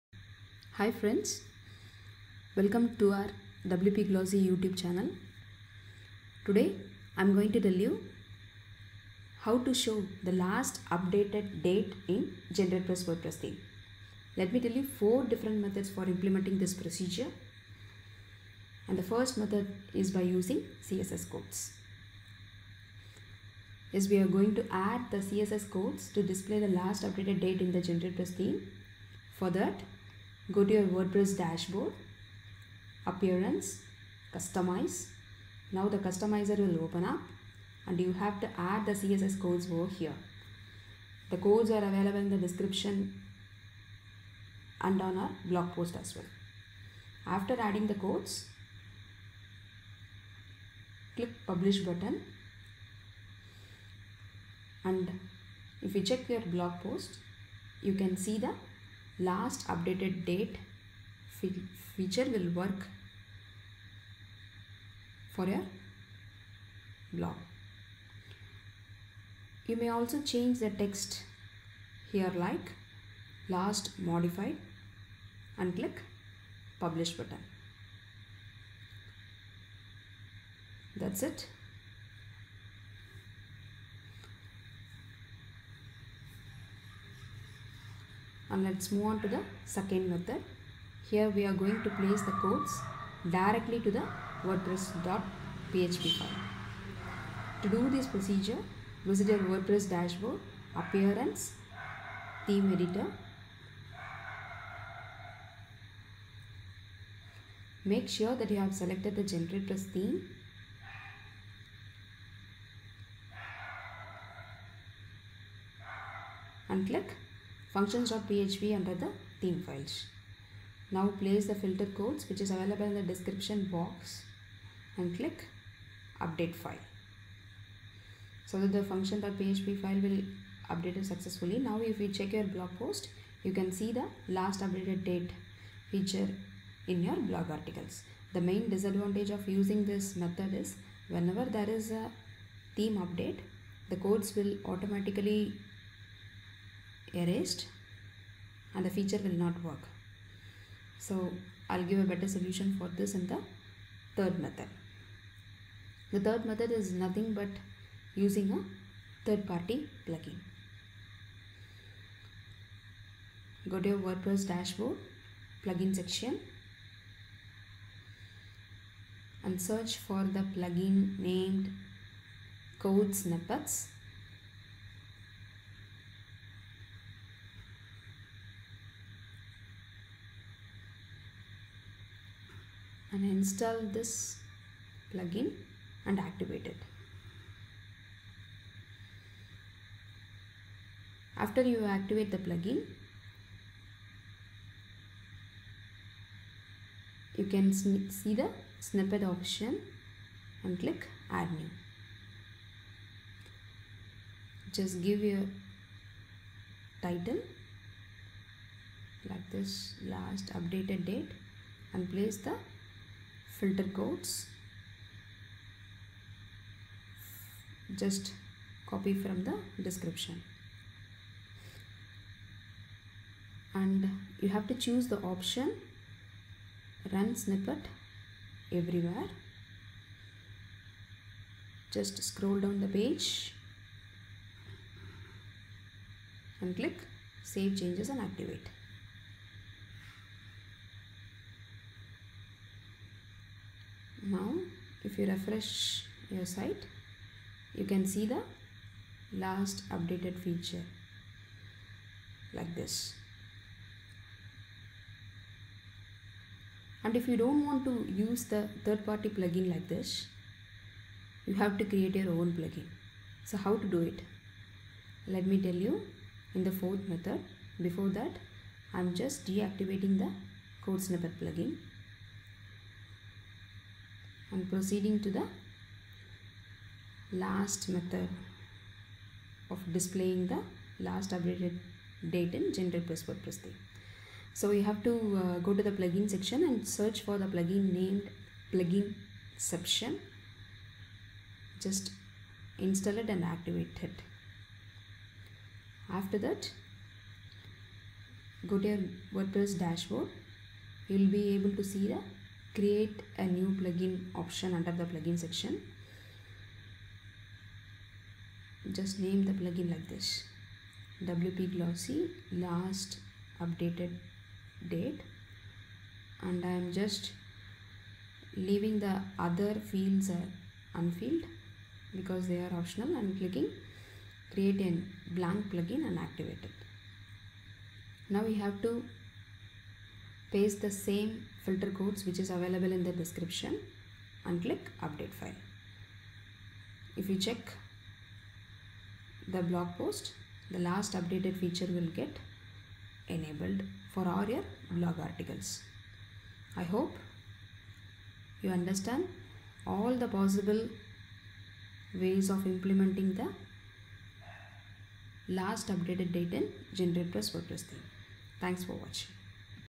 How To Show Last Updated Date In GeneratePress WordPress Theme To Boost Your Google Rankings?
Showing the "Last Updated Date" for every blog post is one of the best SEO strategies that boost your Google rankings.

If you are using the GeneratePress theme for your WordPress site, then you must implement this procedure.

In this video tutorial, I have shared 4 different methods to display the "Last Updated Date" in the GeneratePress theme. You may execute your convenient method.

The disadvantage of using the 2nd method is, whenever there is an update from the theme, the codes will automatically be removed and the feature will not work. So, you have to add the codes again.

So, you may go for the 3rd or 4th method to place the codes permanently.

If you are not using the GeneratePress WordPress theme, then I would suggest you get it and make your site loading super fast.

If you need any help in adding the codes, you may reach us through the blog comment section.